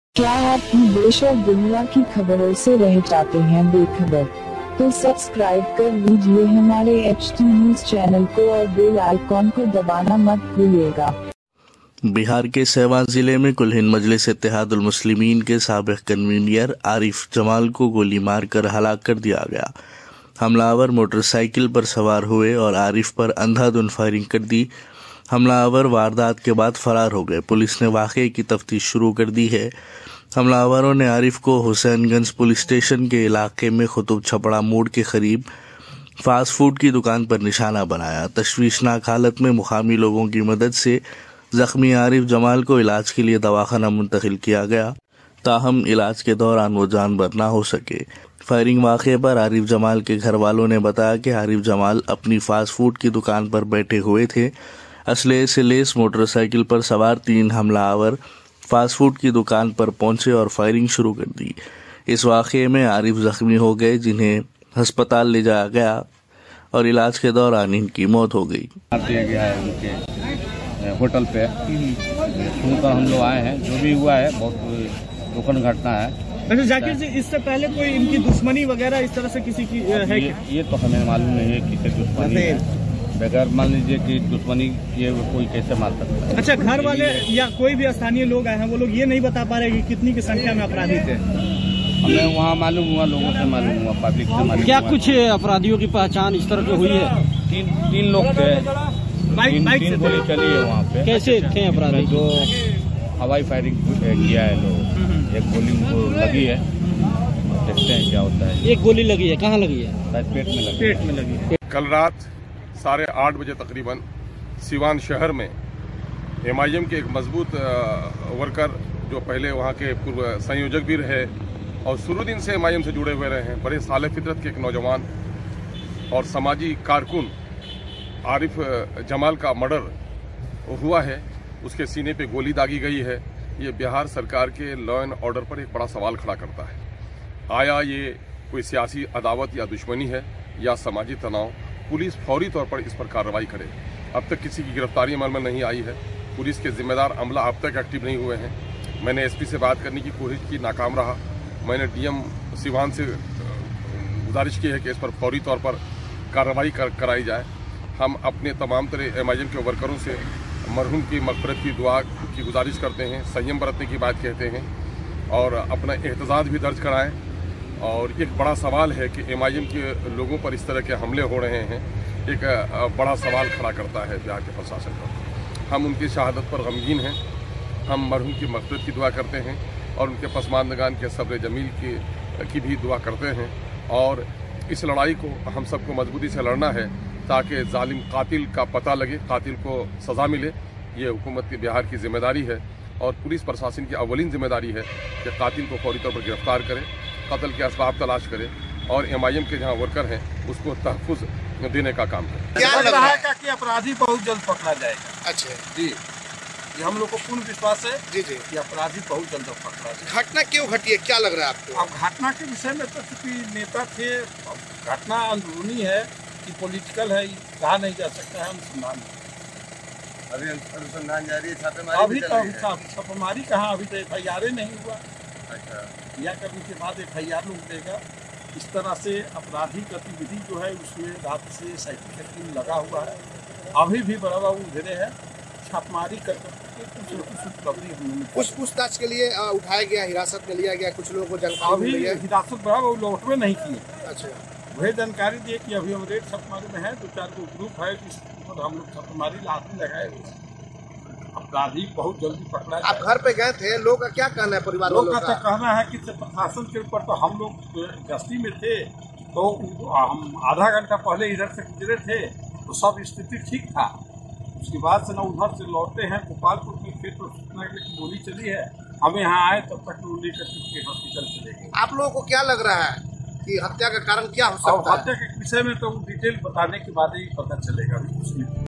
AIMIM leader Arif Jamal shot dead in Bihar's Siwan
AIMIM District President ArifJamal shot dead in Siwan, miscreants entered the restaurant and fired three bullets.
siwan All India Majlis-e-Ittehadul Muslimeen leader Arif Jamal was shot by criminals on Saturday evening in Siwan, Bihar. The people present there at the time of the incident hurriedly took Arif to the hospital for treatment. , He died during treatment. Police is searching for the attackers. Besides, it is also trying to find out what is the reason behind this murder. The District President of Asaduddin Owaisi’s party AIMIM was shot at 7:45 pm on Saturday night by criminals at a fast food shop located near Qutub Chapra Mor in Hussainganj police station area. With the help of local people, injured Arif Jamal was admitted to the hospital for treatment in critical condition. Seeing the serious condition of the AIMIM leader, he was referred to a higher center for better treatment but he would die.

Regarding the incident, family members told that Arif Jamal was sitting at his fast food shop. Three criminals riding a bike armed with weapons reached the fast food shop and opened fire indiscriminately, causing injuries. The criminals fled after committing the incident. Eyewitnesses said that three rounds of bullets were fired by the criminals. The reasons for the incident could not be known. Since the condition was serious, the doctor referred him to PMCH. But he died on the way.